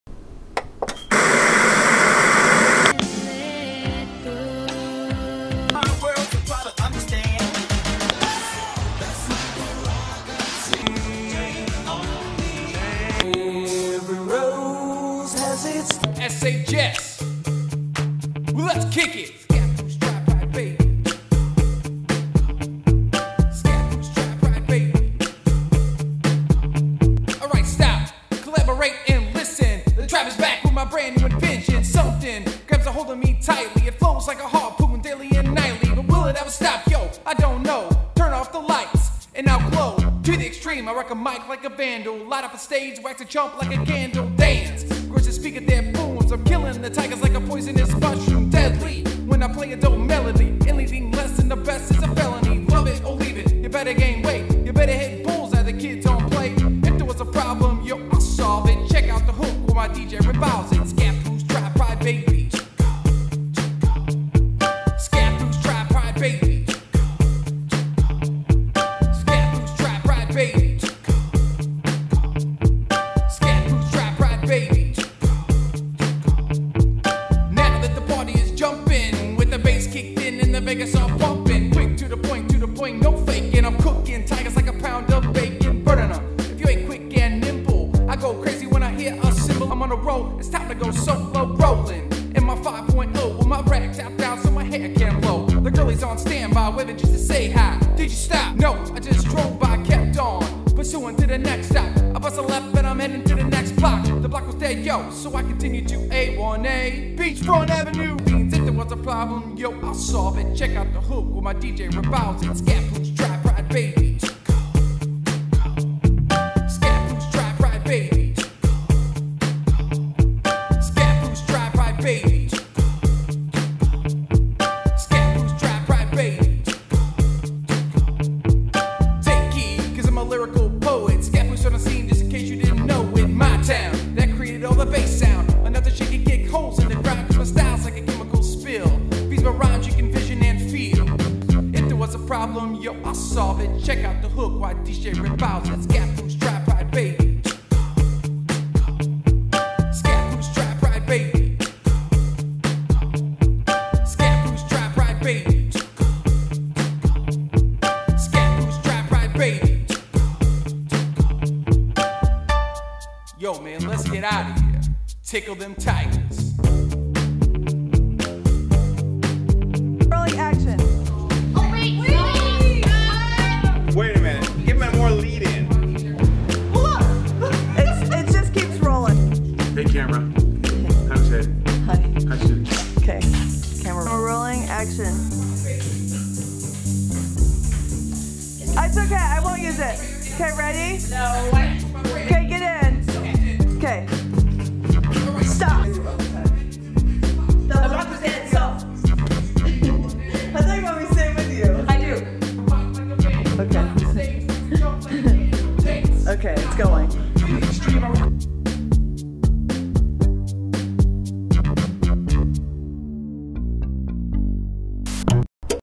Tribe Pride Baby
Scappoose High School Teachers Homecoming Skit: Tribe Pride Baby!